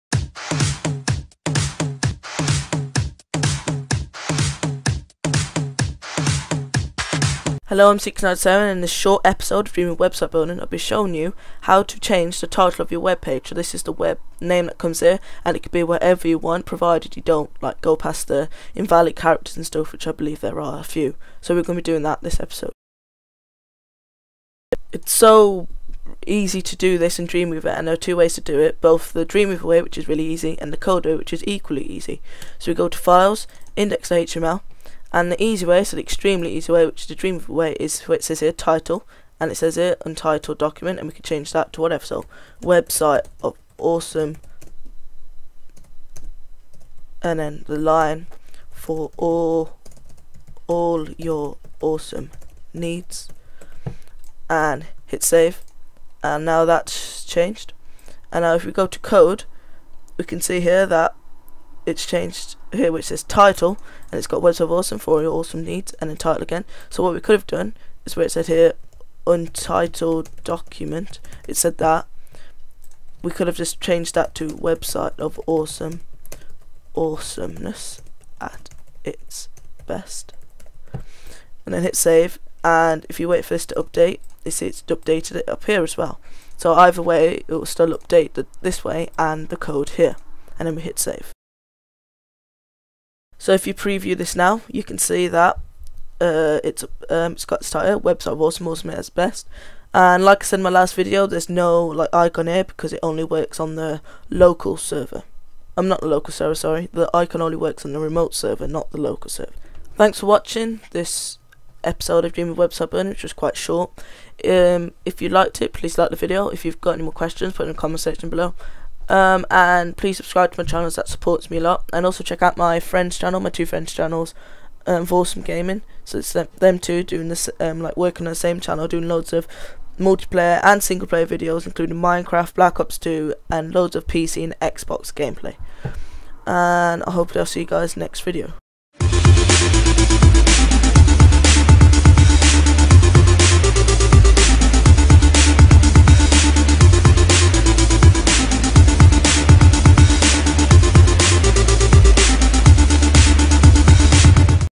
Dreamweaver Website Building - Dreamweaver Website Building / #11 ~ Changing The Title Of A Page
eamweaver Website Building Episode 11. I show you how to change the title of a page, the name of it in a tab next to the shortcut icon.